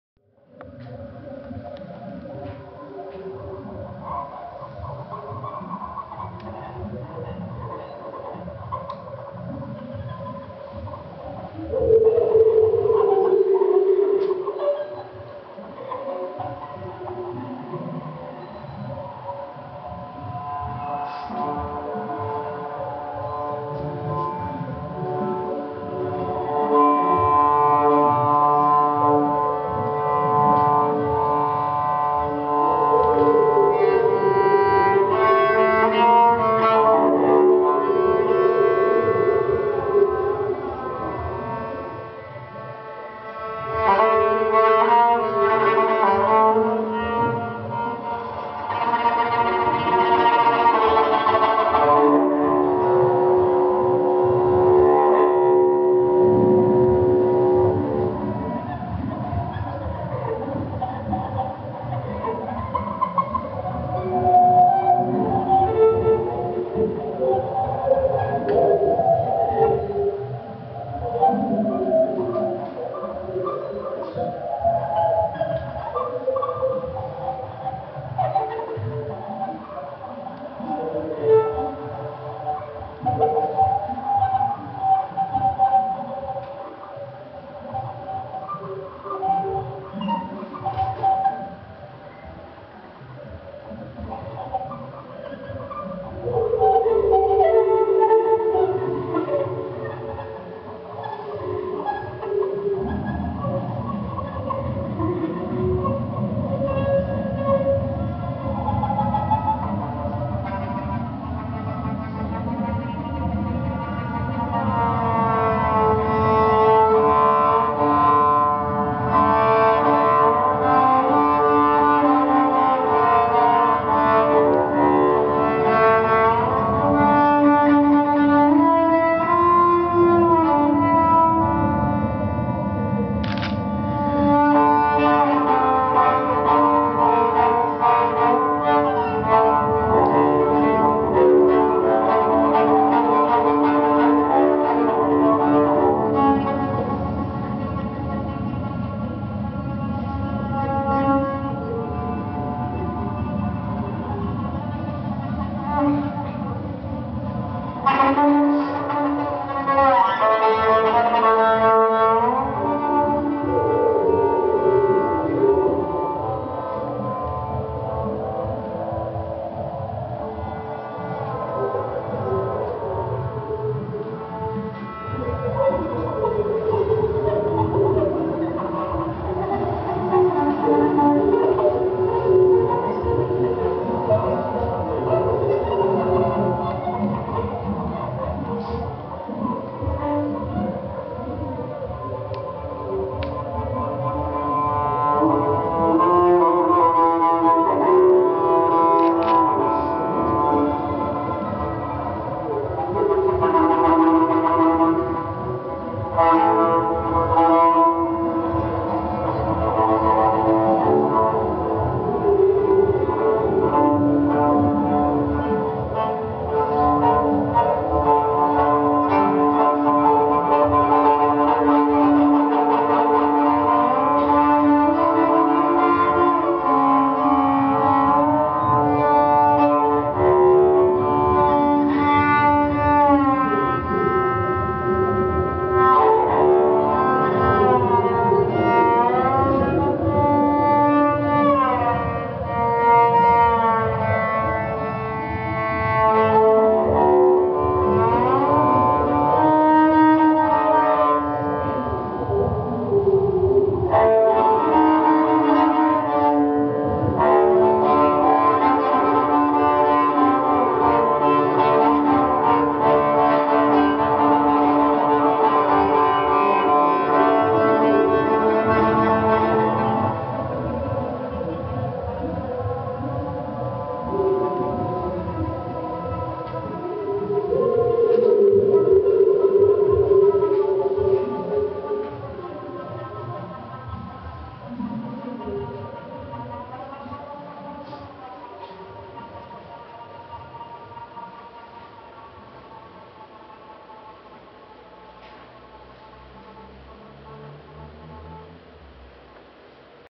Concert "Νερό» by International Sound Artist Ros Bandt
A sonic celebration of the world's most precious substance
featuring
International Sound Artist Ros Bandt
as composer/performer, with tarhu flutes, environmental soundscapes, and video.

This video is the seventh piece of her performance.

This concert took place at the Athens Centre, on Wednesday October 19, 2011.